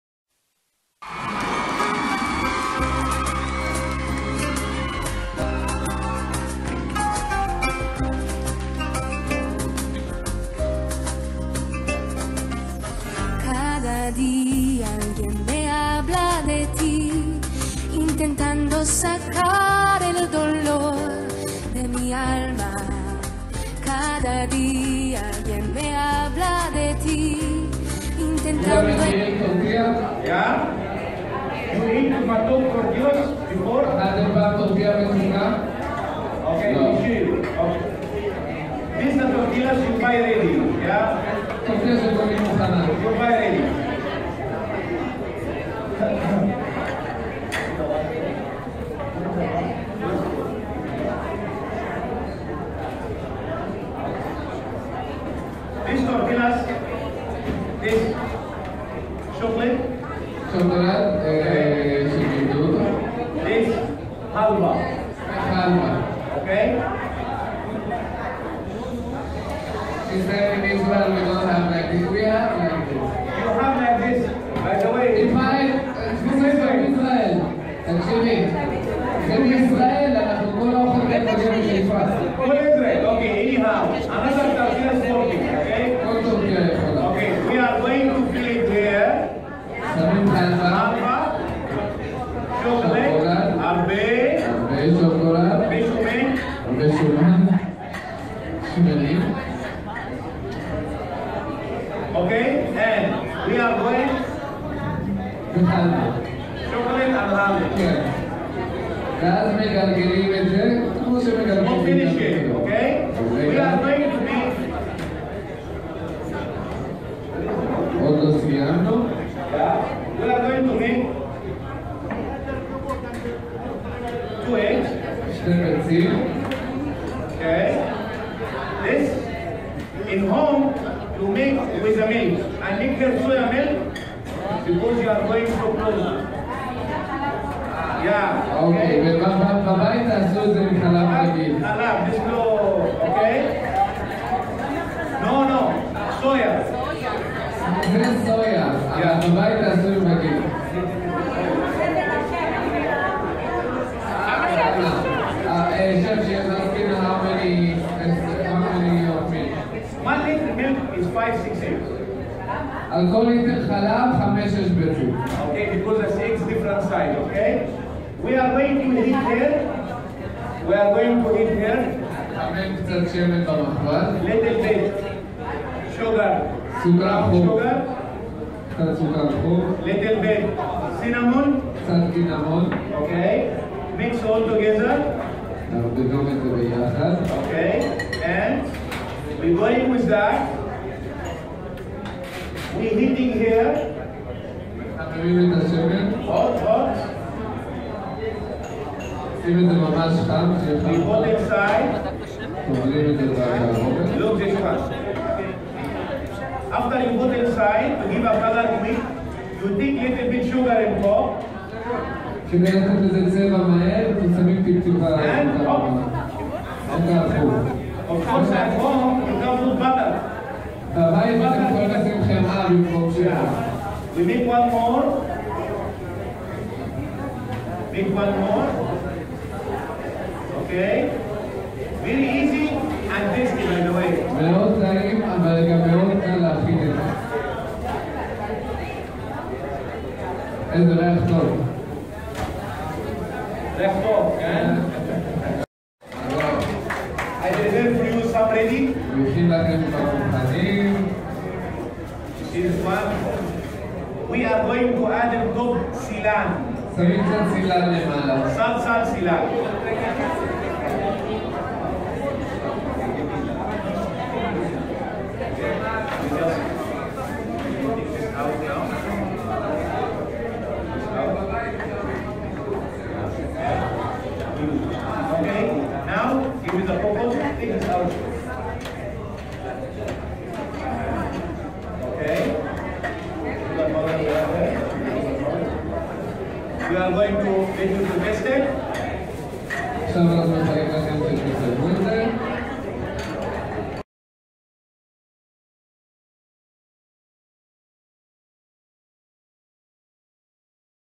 הכנת טורטייה
אופן הכנת טורטייה. הדגמה: השף האני. מנו ספנות.
צילם: משה דולין 4.5.2017